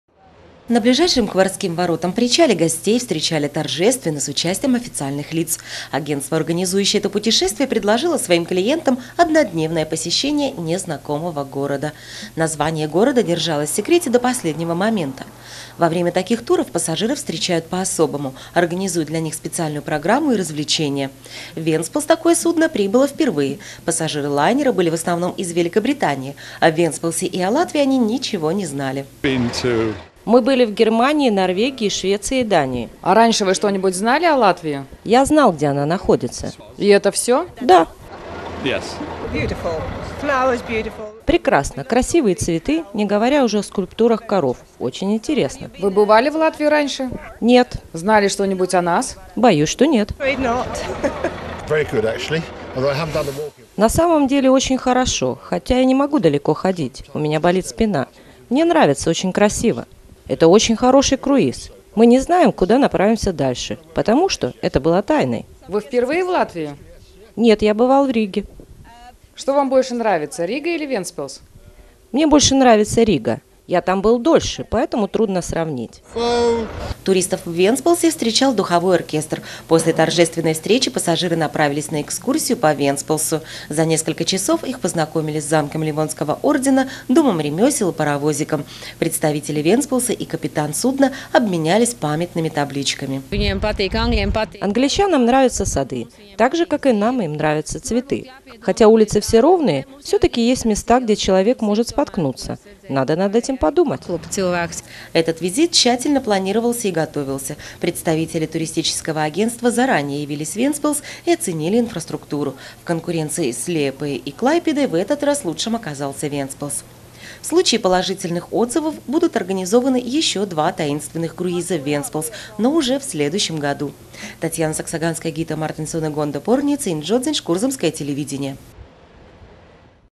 В Вентспилсский порт заходит круизное судно
Работники Вентспилсского информационно-туристического центра  на причале Вентспилсского порта № 37 (рядом с кораблем "Азова") встретили около 650 пассажиров судна "Таинственного круиза", чтобы ознакомить их с богатым предложенияем, подготовленным городом Вентспилс в области туризма. Судно "Таинственного круиза" в Вентспилсский порт зашел впервые. Круиз организован компанией "Riga Cruiseship Agency", которая своим клиентам предлагает возможность один день провести в заранее неизвестном портовом городе, название которого держится в тайне до последнего момента.